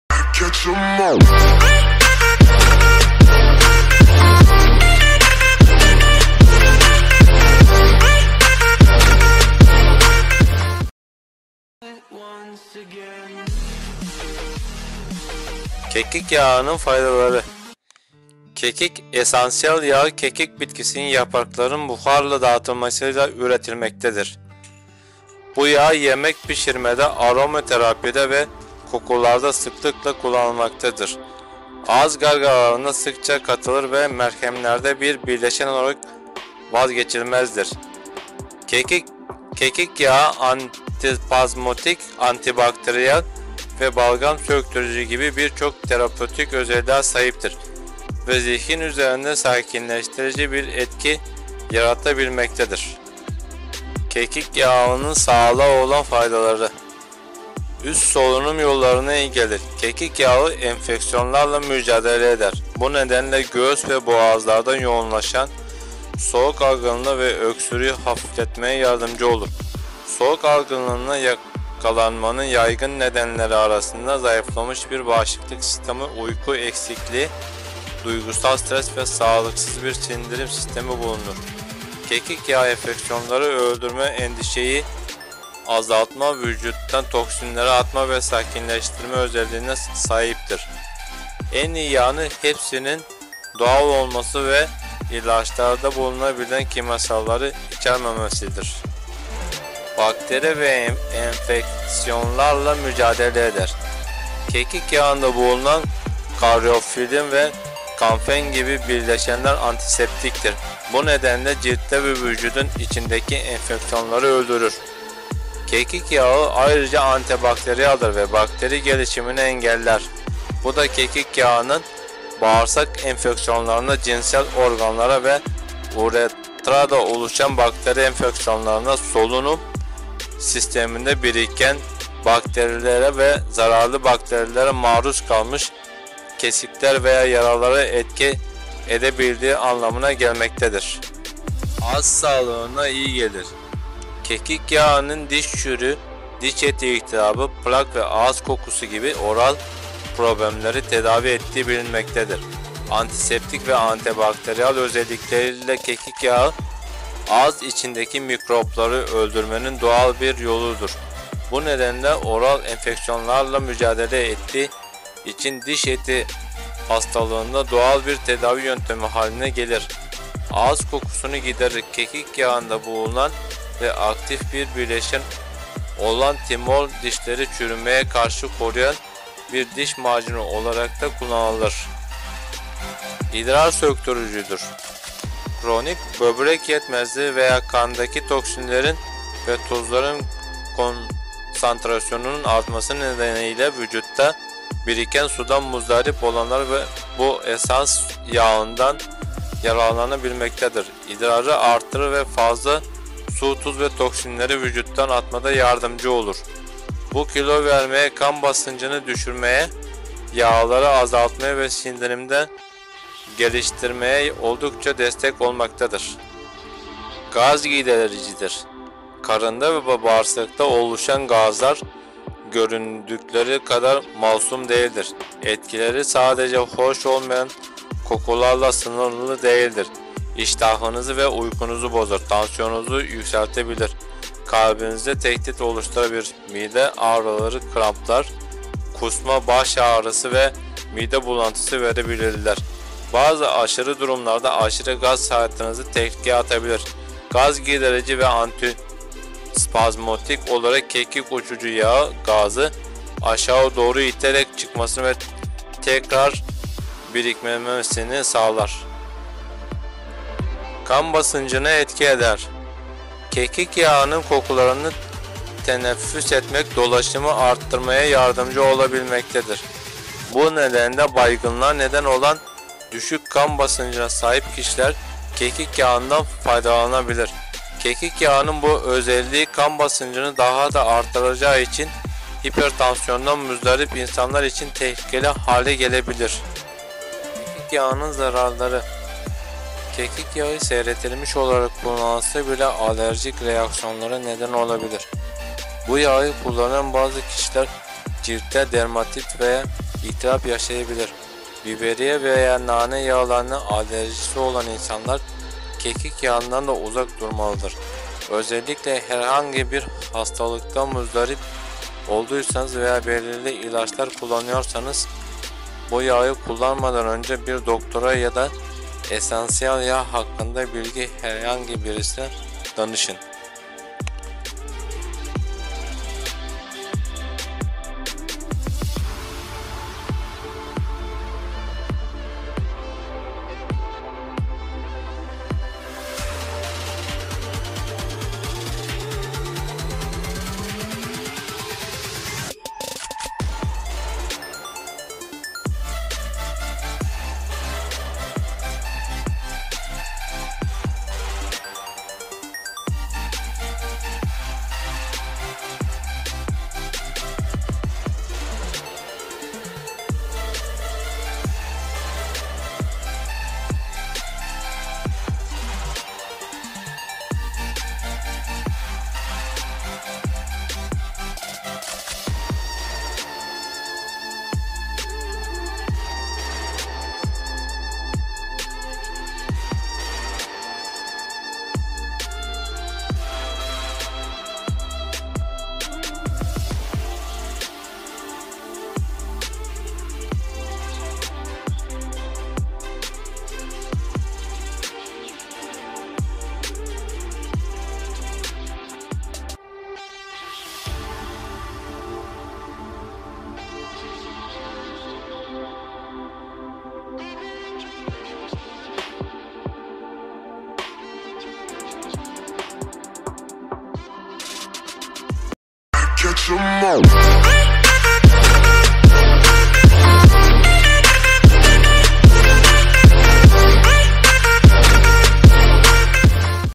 Kekik Yağı Antiseptik ve Antibakteriyel özellikleri
Kekik esansiyel yağı, kekik bitkisinin yapraklarının buharla damıtılmasıyla üretilmektedir. Bu yağ yemek pişirmede, aroma terapide ve kokularda sıklıkla kullanılmaktadır. Ağız gargaralarına sıkça katılır ve merhemlerde bir bileşen olarak vazgeçilmezdir. Kekik yağı antispazmodik, antibakteriyel ve balgam söktürücü gibi birçok terapötik özelliğe sahiptir ve zihin üzerinde sakinleştirici bir etki yaratabilmektedir
Kekik yağının sağlığa olan faydaları
Üst solunum yollarına iyi gelir: Kekik yağı enfeksiyonlarla mücadele eder. Bu nedenle, göğüs ve boğazda yoğunlaşan soğuk algınlığı ve öksürüğü hafifletmeye yardımcı olur. Soğuk algınlığa yakalanmanın yaygın nedenleri arasında zayıflamış bir bağışıklık sistemi, uyku eksikliği, duygusal stres ve sağlıksız bir sindirim sistemi bulunur. Kekik yağı enfeksiyonları öldürme, endişeyi azaltma, vücuttan toksinleri atma ve sakinleştirme özelliklerine sahiptir. En iyi yanı, hepsinin doğal olması ve ilaçlarda bulunabilen kimyasalları içermemesidir.
Bakteri ve enfeksiyonlarla mücadele eder: Kekik yağında bulunan karyofilin ve kamfen gibi bileşenler antiseptiktir. Bu nedenle, ciltte ve vücudun içindeki enfeksiyonları öldürür. Kekik yağı ayrıca antibakteriyeldir ve bakteri gelişimini engeller. Bu da kekik yağının bağırsak enfeksiyonlarına, cinsel organlarda ve üretrada oluşan bakteri enfeksiyonlarına, solunum sisteminde biriken bakterilere ve zararlı bakterilere maruz kalmış kesikler veya yaralara etki edebildiği anlamına gelmektedir.
Ağız sağlığını iyi gelir: Kekik yağının, diş çürüğü, diş eti iltihabı, plak ve ağız kokusu gibi oral problemleri tedavi ettiği bilinmektedir. Antiseptik ve antibakteriyel özellikleri ile kekik yağı, ağız içindeki mikropları öldürmenin doğal bir yoludur. Bu nedenle oral enfeksiyonlarla mücadele ettiği için diş eti hastalığında doğal bir tedavi yöntemi haline gelir. Ağız kokusunu giderir. Kekik yağında bulunan ve aktif bir bileşen olan timol, dişleri çürümeye karşı koruyan bir diş macunu olarak da kullanılır
Romatizma ile mücadele eder: Romatizma, artrit ve gutun arkasında iki ana neden vardır. Birincisi dolaşımın yetersiz ya da tıkalı olması, ikincisi ise kan akışında ürik asit gibi toksinlerin konsantrasyonunun artmasıdır. Kekik esansiyel yağı, bu sorunların her ikisini de çözebilir. Bir diüretik olduğundan idrarı arttırır ve vücuttan toksinlerin atılmasına yardımcı olur. Kekik yağı dolaşımı uyarır veya etkinleştirir. Böylece her iki şekilde de etkili olarak romatizma ve artrit gibi ilgili durumları iyileştirmeye destekçi olur.
İdrar söktürücüdür: Kronik böbrek yetmezliği veya kandaki toksinlerin ve tuzların konsantrasyonunun artması nedeniyle vücutta biriken sudan muzdarip olanlar bu esans yağından yararlanabilmektedir. İdrarı arttırır ve fazla su, tuz ve toksinleri vücuttan atmada yardımcı olur. Bu kilo vermeye, kan basıncını düşürmeye, yağları azaltmaya ve sindirimi de geliştirmeye oldukça destek olmaktadır
Gaz gidericidir: Karında ve bağırsakta oluşan gazlar göründükleri kadar masum değildir. Etkileri sadece hoş olmayan kokularla sınırlı değildir. İştahınızı ve uykunuzu bozar, tansiyonunuzu yükseltebilir, kalbinize tehdit oluşturabilir, mide ağrıları, kramplar, kusma, baş ağrısı ve mide bulantısı verebilirler. Bazı aşırı durumlarda, aşırı gaz hayatınızı tehlikeye atabilir. Gaz giderici ve antispazmodik olarak kekik uçucu yağı gazı aşağı doğru iterek, çıkmasını ve tekrar birikmemesini sağlar
Kekik yağı seyreltilmiş olarak kullanılsa bile alerjik reaksiyonlara neden olabilir. Bu yağı kullanan bazı kişiler ciltte dermatit veya iltihap yaşayabilir. Biberiye veya nane yağlarına alerjisi olan insanlar kekik yağından da uzak durmalıdır. Özellikle herhangi bir hastalıktan muzdarip olduysanız veya belirli ilaçları kullanıyorsanız, bu yağı kullanmadan önce bir doktora ya da
esansiyel yağlar hakkında bilgili herhangi birisine danışın
Tavsiyeler
Yorgunluğu gidermek için banyo suyuna 2 damla kekik yağı ekleyebilirsiniz.
Menstrüel krampları gidermek için 2 damla kekik yağı ve eşit miktarda taşıyıcı yağ ile karnınızı ovabilirsiniz.
Bir gargara olarak kullanmak için, suya 2 damla kekik yağı ekleyip gargara yapabilirsiniz.
Tıkanmış burun kanallarını açmak için, 2 damla kekik yağı soluyabilirsiniz veya buharın teneffüs edilmesi için sıcak suya ekleyebilirsiniz.
Ayak mantarını öldürmek için, sıcak su ile dolu olan ayak banyosuna 5 damla kekik yağı ekleyebilirsiniz.
Enfeksiyonları ve kızarıklıkları öldürmek için, 2 damla kekik yağını gereken bölgeye uygulayabilirsiniz.
Dolaşımı artırmak için, günlük olarak 2-3 damla kekik yağını soluyabilir ya da vücudunuza sürebilirsiniz.